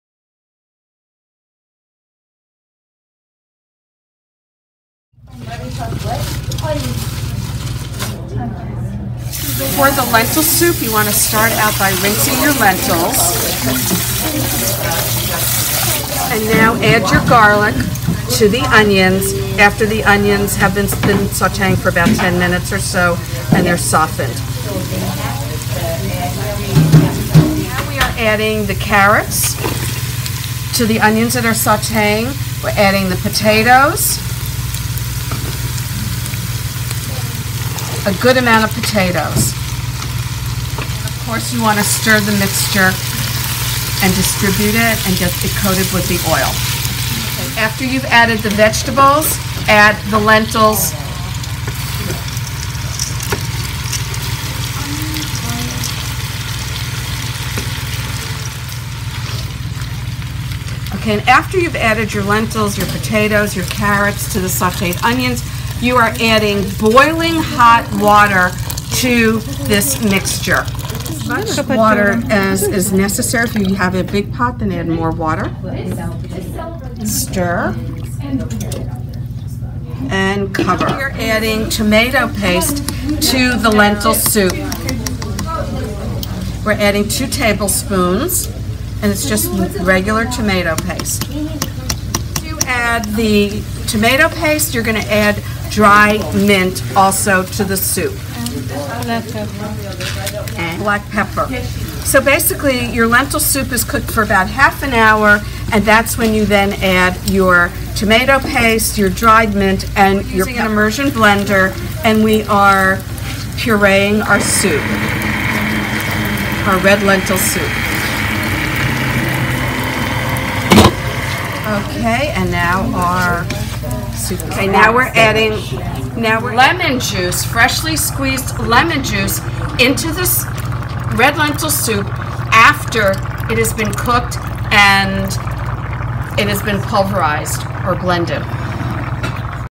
Turkish Red Lentil Soup | Mercimek Çorbası
Ingredients:
1 cup red lentils, rinsed and drained
3 tablespoons olive oil / vegetable oil
2 tablespoons tomato paste 1 onion, diced
1 potato, grated
1 carrot, grated
Directions:
5 cups hot water
1 clove garlic
1 tablespoon lemon juice 1 tablespoon salt
1 teaspoon black pepper 1 tablespoon butter
Red pepper flakes
Dried mint
Red Lentil Soup – Mercimek Çorbası
               Heat the olive oil in a large pot. Add onions and sauté for about 2 minutes. Then add grated carrots and potatoes as well as rinsed and drained red lentils. Cook 4-5 minutes over low heat (if needed add 1⁄4 cup of hot water to prevent vegetables from burning). Add the hot water, juice of a lemon and let it simmer stirring occasionally until lentils are very soft. Season with salt and pepper. Puree the soup with a blender. Prepare a sauce for garnishing the soup: melt the butter, add dried mint and red pepper flakes, and cook for a minute until butter is infused by the spices. Pour over the soup. If the soup is too thick, add some hot water to it.